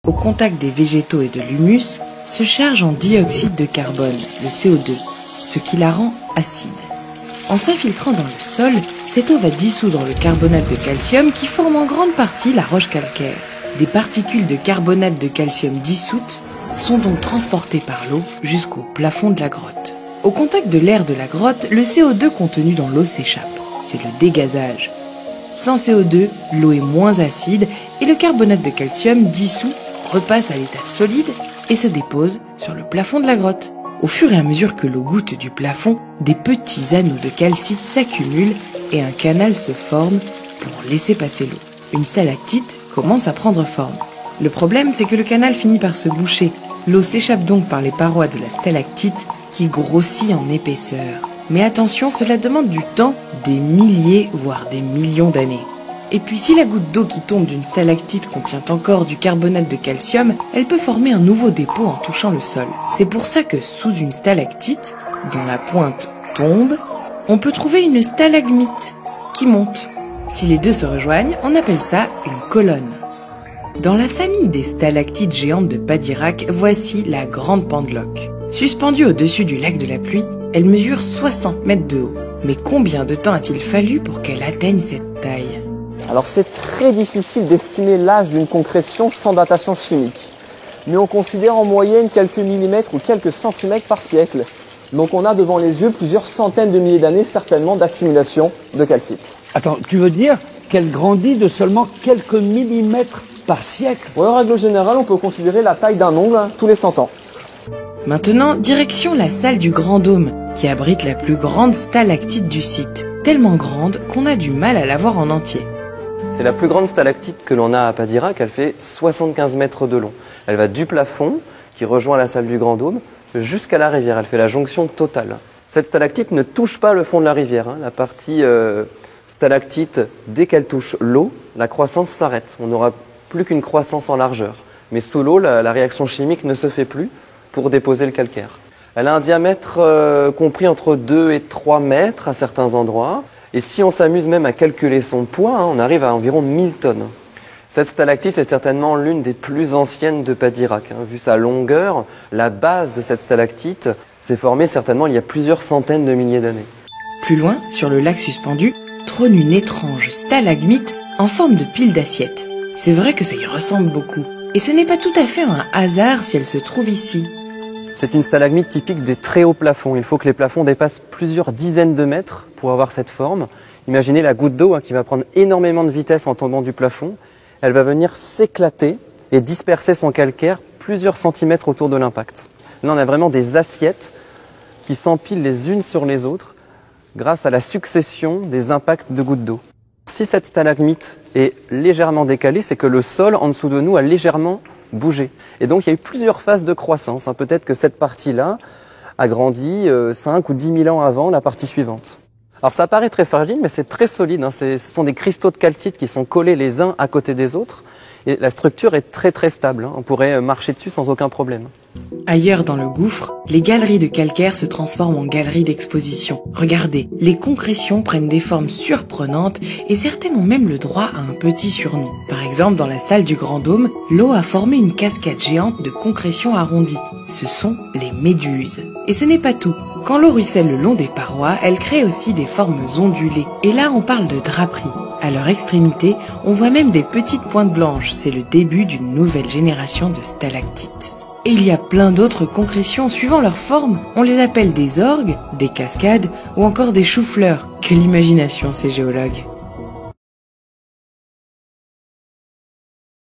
Stalactites et stalagmites
Présentation et différences entre les stalactites et les stalagmites